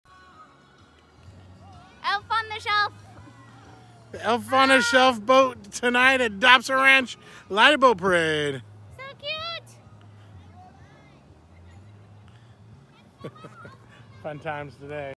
Elf On The Shelf Boat
The original date got rained out, so ￼ this year’s boat parade was a lot more casual, and a lot less crowded.